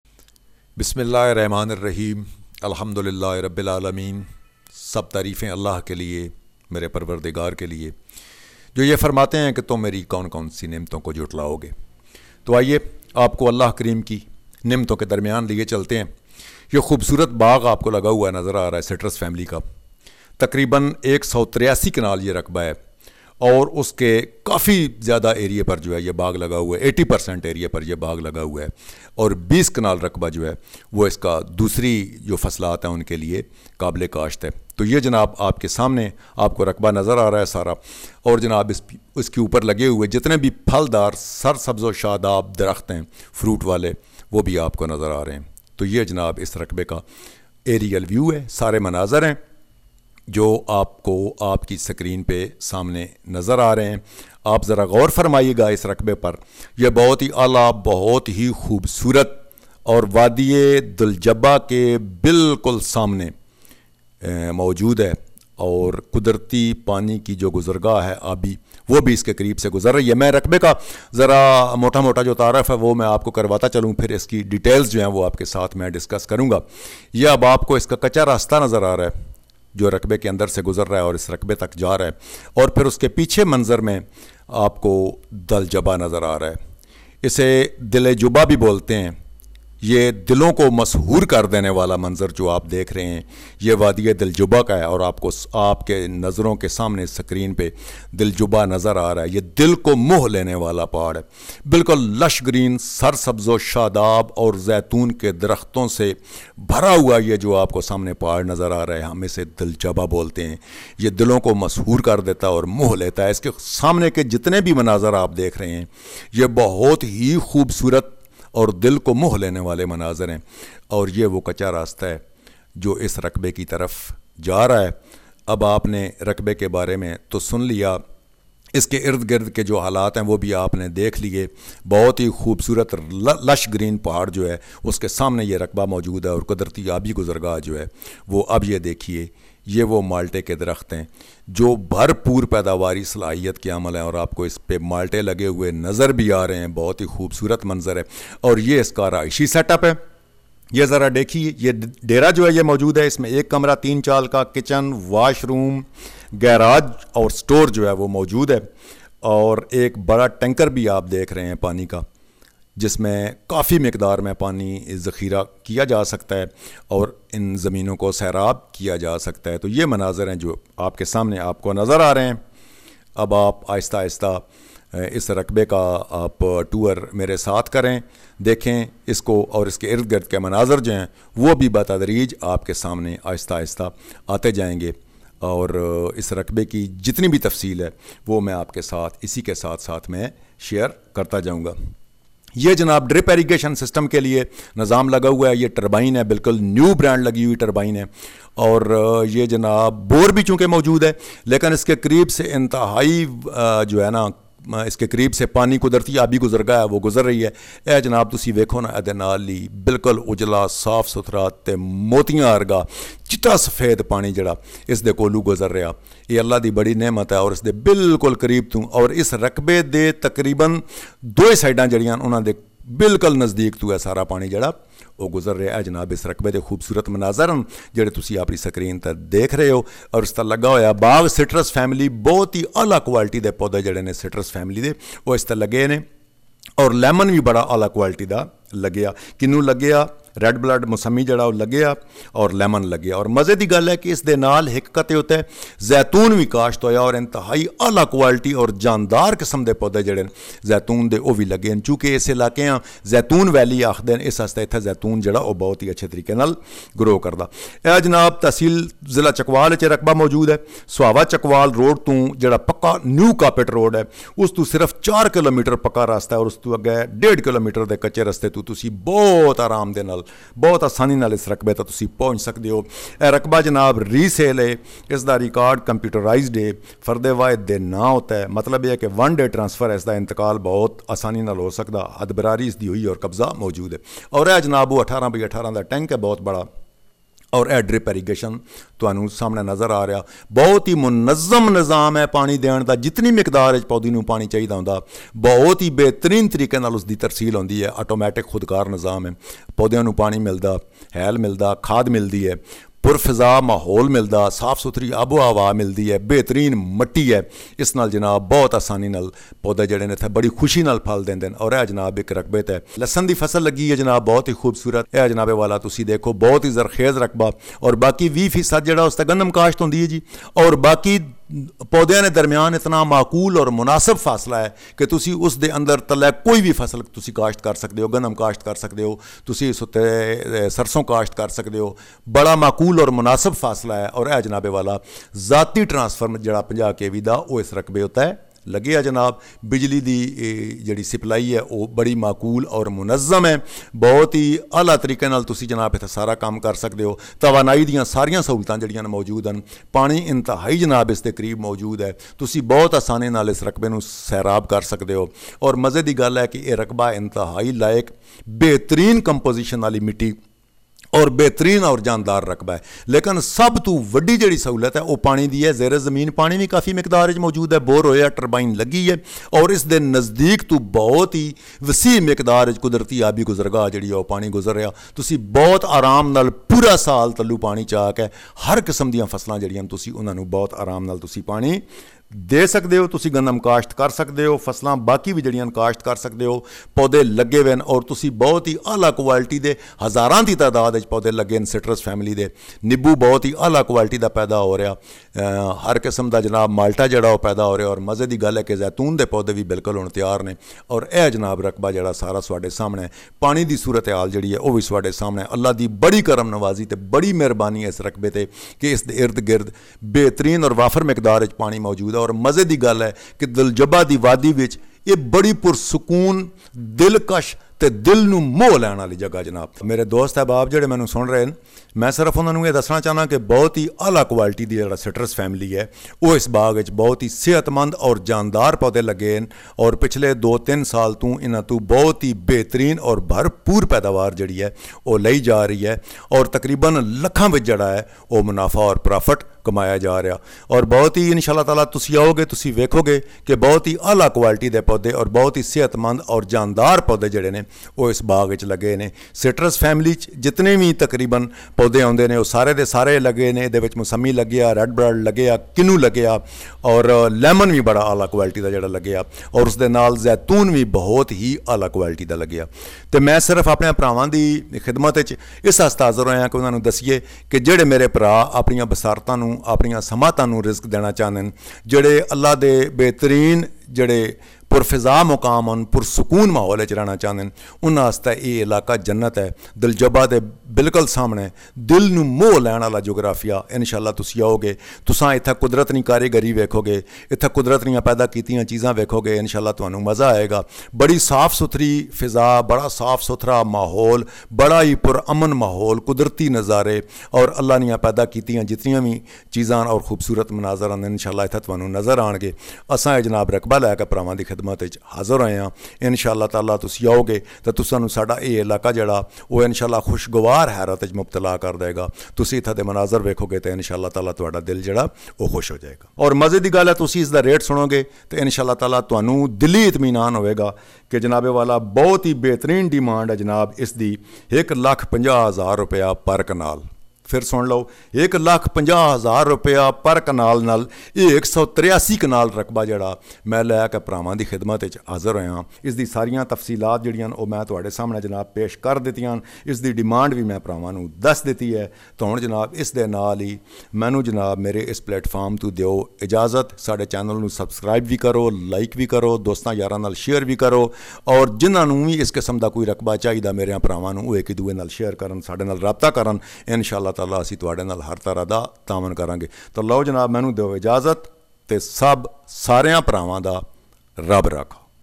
183 Kanal Farm House For Sale District Chakwal | On Chakwal To Sohawa Main GT Road.
2 Kanal house for sale

183 Kanal Farm House For Sale District Chakwal |  Agricultural Land For Sale | Zari Raqba  | On  Chakwal To Sohawa Main GT Road.